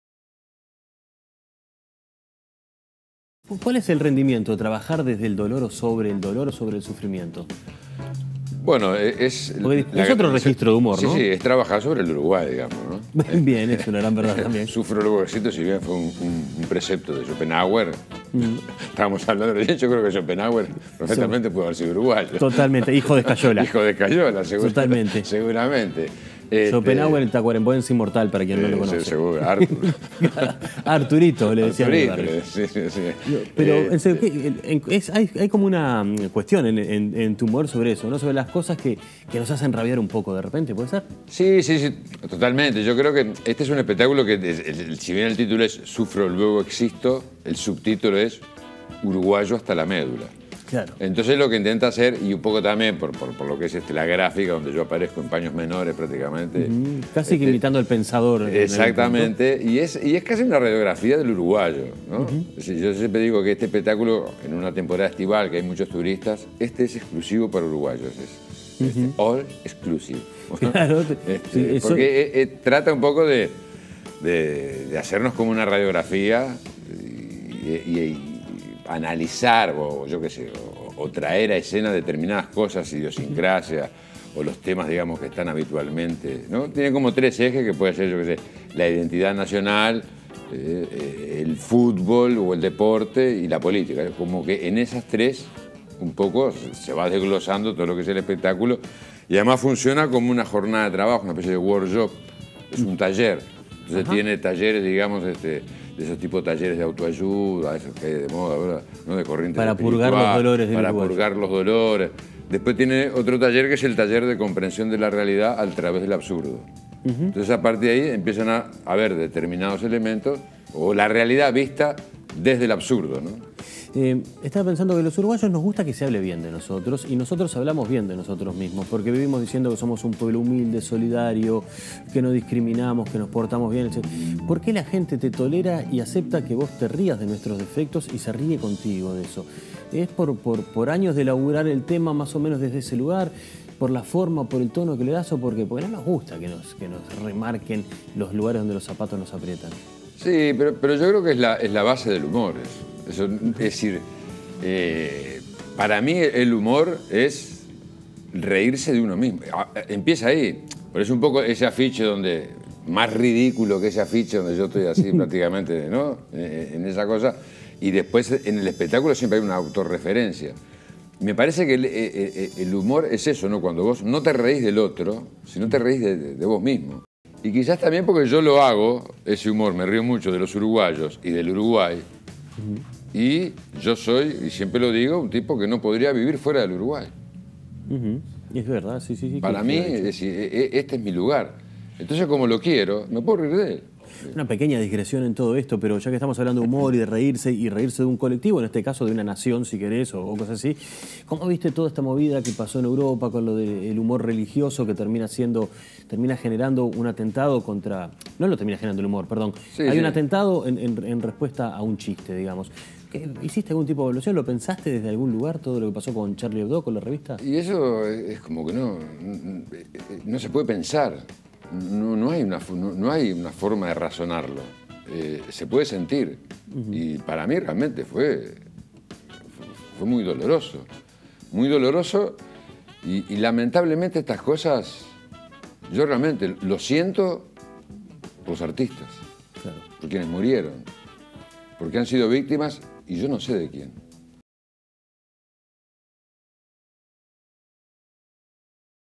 Jorge Esmoris estrena “Sufro, luego existo”
Conversamos con este uruguayo hasta la médula sobre su nuevo monólogo, el sufrimiento autóctono y su sentimiento con los integrantes de Charlie Hebdo.

“Sufro, luego existo: uruguayo hasta la médula” es el título del unipersonal que Jorge Esmoris estrena el sábado 31 de enero en la Vieja Farmacia Solís. El eterno sufrir de los uruguayos, las libertades que se le dan a la hora de hacer humor y hasta el atentado contra la revista francesa Charlie Hebdo formaron parte de esta charla de Álvaro Carballo con el actor.